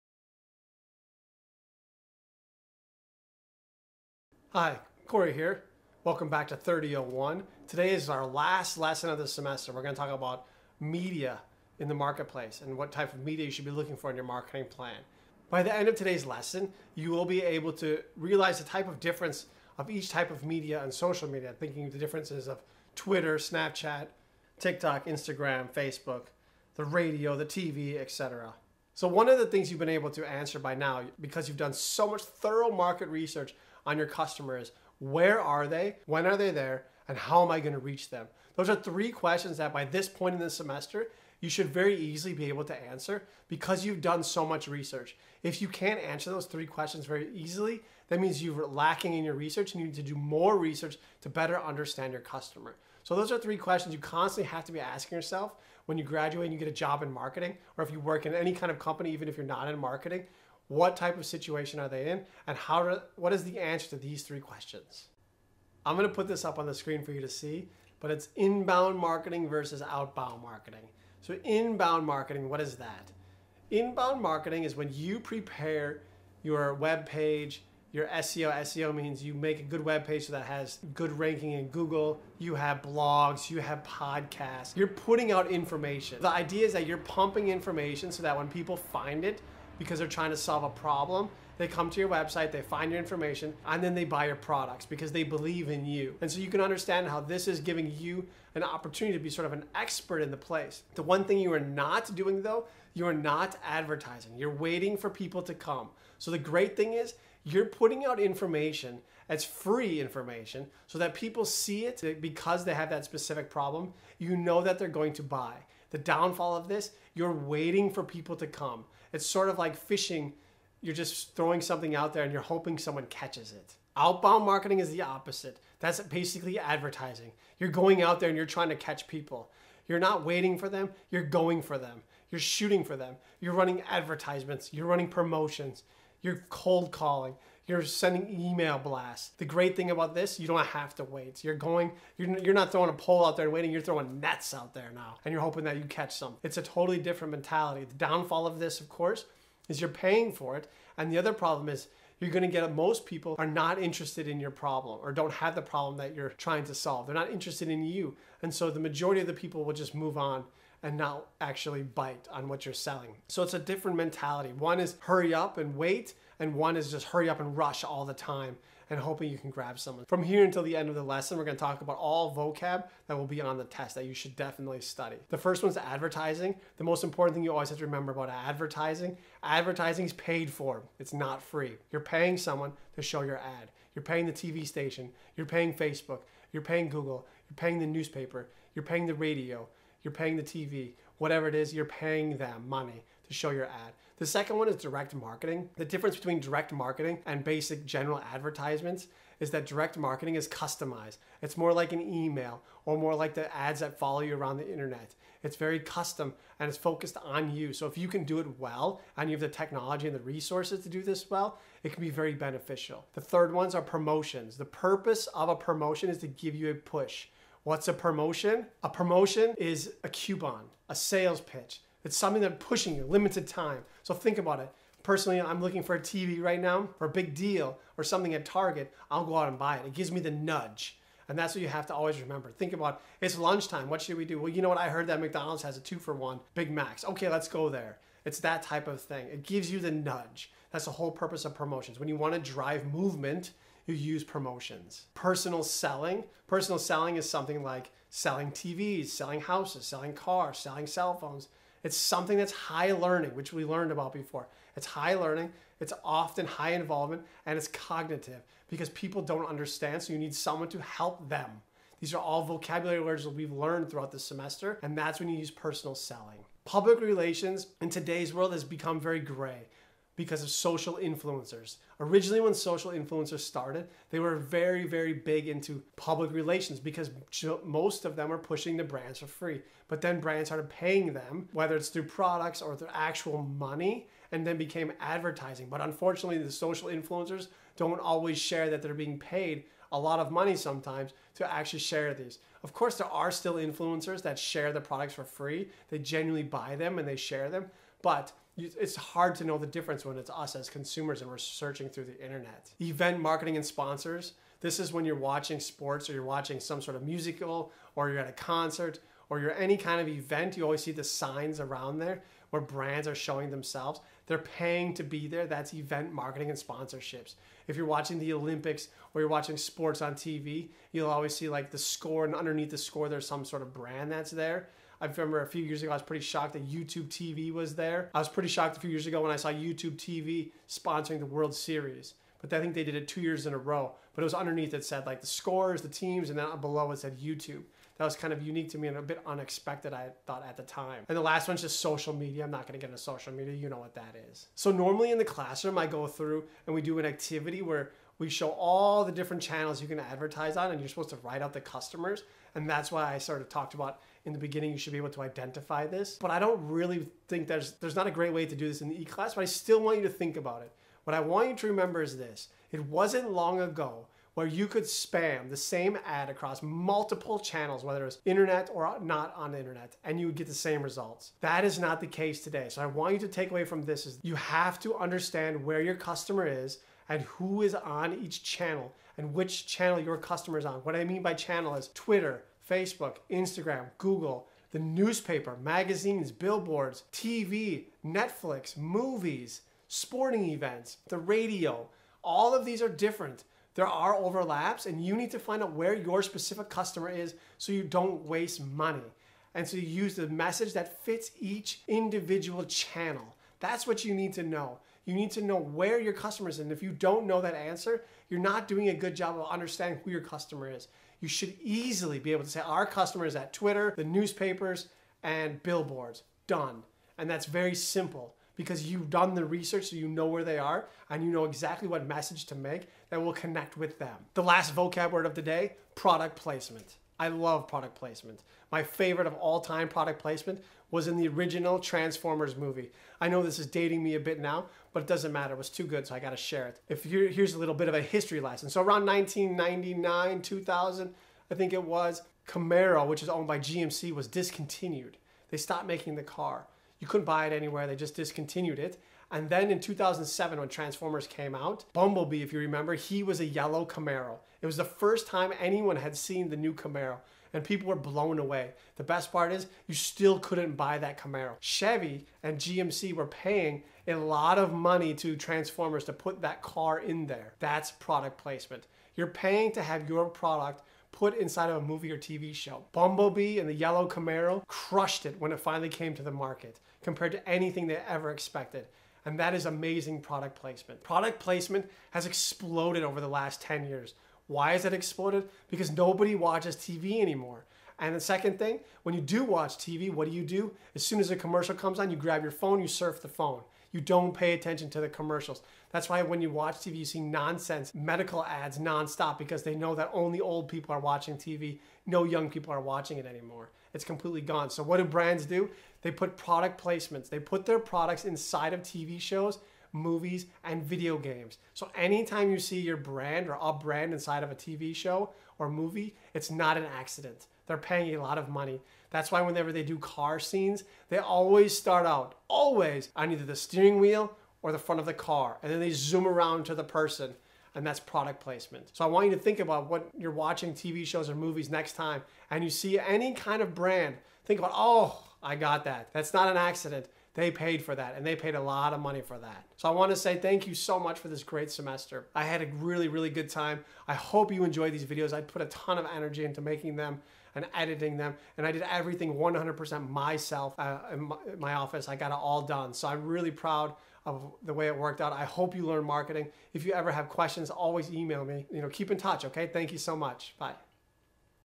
Marketing Communication: Media
Decisions made about media define where, when, and how the message will be delivered. Consumers encounter and respond to marketing communications in two different ways: they are either passively exposed to ads and promotions or they actively seek them out. This difference forms the basis for separating communication strategies into inbound and outbound marketing.